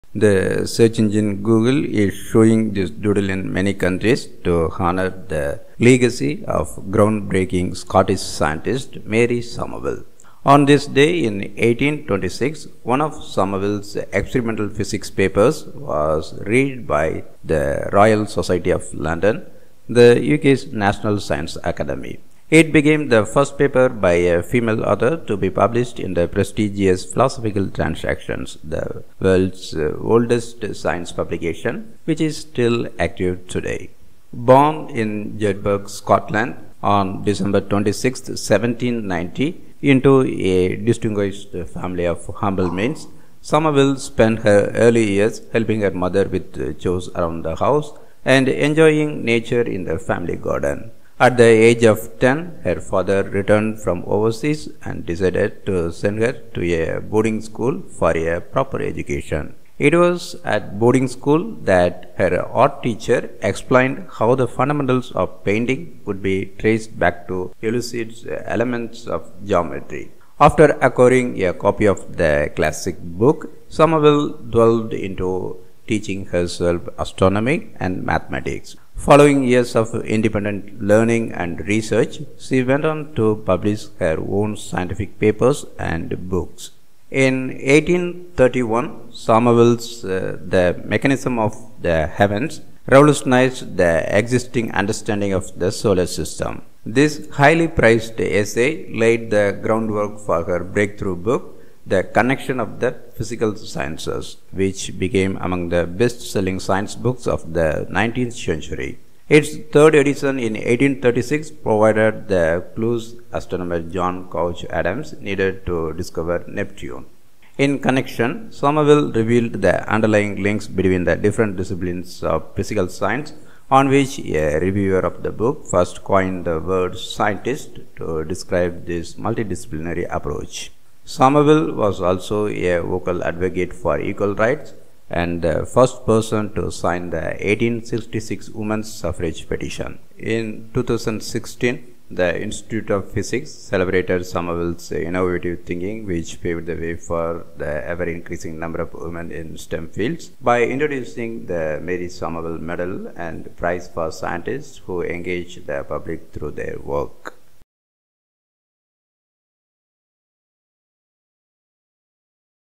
Mary Somerville Google Doodle
Google is showing a Doodle in many countries to honor the legacy of groundbreaking Scottish scientist Mary Somerville. On this day (2nd February) in 1826, one of Somerville’s experimental physics papers was read by the Royal Society of London, the UK’s National Science Academy. It became the first paper by a female author to be published in the prestigious Philosophical Transactions, the world’s oldest science publication, which is still active today.